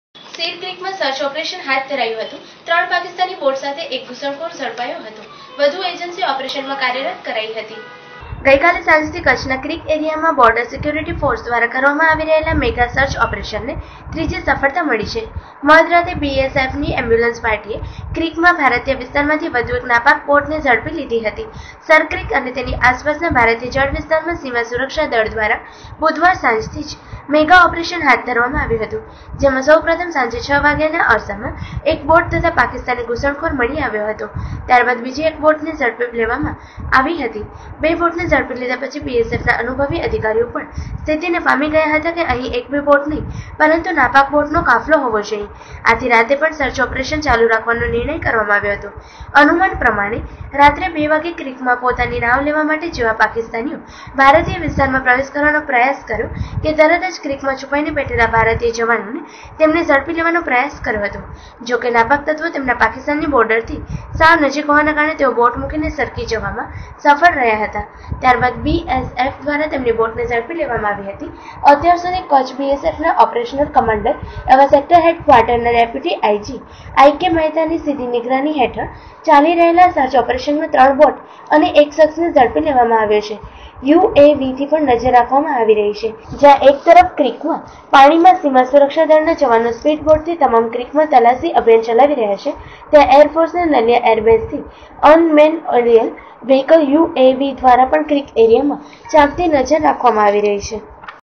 સીર ક્રીકમાં સર્ચ ઓપરેશન હાથ ધરાયું વિજય ઘેલાણી (આમતક ન્યુઝ, ભુજ) મો. ૯૭૧૨૯૯૭૩૦૯
Kutch Aamtak News Bhuj Dt.19-4-18